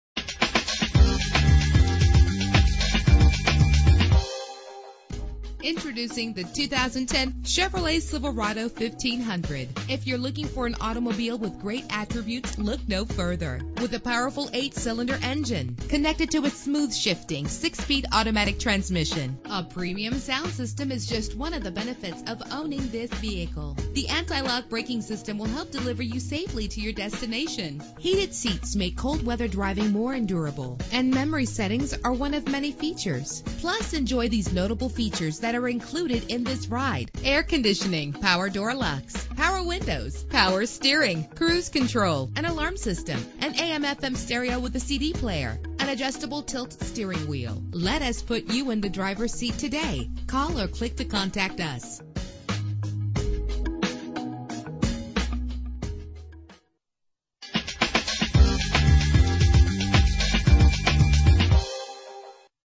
2010 Chevrolet Silverado 1500 - PEARLAND TX
Year: 2010
Make: Chevrolet
Model: Silverado 1500
Trim: LTZ
Engine: 5.3 liter 8 cylinder 16 valve
Transmission: 6-Speed Automatic
Color: Victory Red
Mileage:
Address:
5719 BROADWAY
PEARL